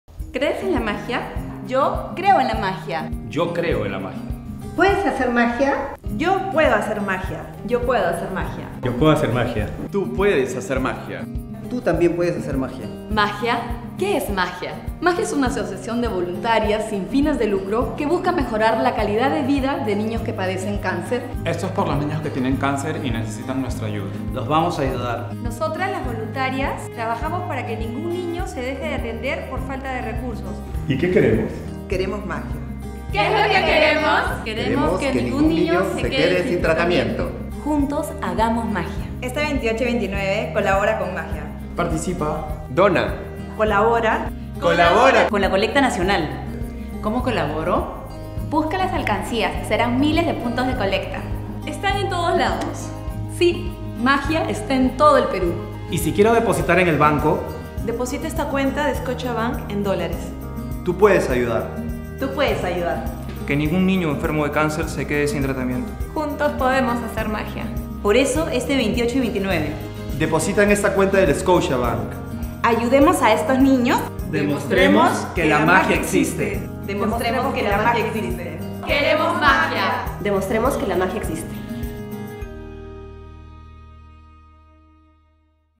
Crees en la Magia? Parte 2
La industria de la moda en el Perú se pone de pie para hacer Magia! Este 28 y 29 de mayo colabora con la colecta Nacional! Por los niños que tienen cáncer..

Créditos:
FIlm por Pandemia Films
Dirección creativa por Fashion Society
Make up Artist Ale Chavez
Fotografia de Kike Sanchez
Backstage de Jonathan Jacobs

Agradecimiento especial: Alexander Callegari, Ana María Guilfo, Andrea Rivera, Antonia del Solar, Antonio Choy Kai, Carlos Vigil, Cristina Rendón, Claudia Benavides, Claudia Jiménez, Efrain Salas, Fátima Arrieta, Fernando Torrejón, Gerardo Larrea, Jack Abugattas, Jessica Newton, Jessica Butrich, Jessica Vasquez, Maria Lucia Privat, Joaquin H. Palacin, José Miguel Valdivia, José Clemente, Lorena Larrieviere, Maple Sam, Marina Mora, Meche Correa, Micaela Galvez, Noe Bernacelli, Omar Valladolid, René Funk, Richard Dulanto, Rosario de Armenteras, Sergio Dávila, Sitka Semsch, Stefanie Schiller, Soledad Valenzuela, Sumy Kujon, Titi Guilfo,  Vanessa Rodriguez, Yirko Sivirich
(Miss Perú TOP10: Ornella Pesceros, Marsi Fernandez-Maldonado, Luz Melissa Cartagena, Laura Spoya, Paola Rosenberg, Lorena Larriviere, Claudia Manrique, Kayla Abensur, Jessamin Chaparro, y Leonela Alzamora)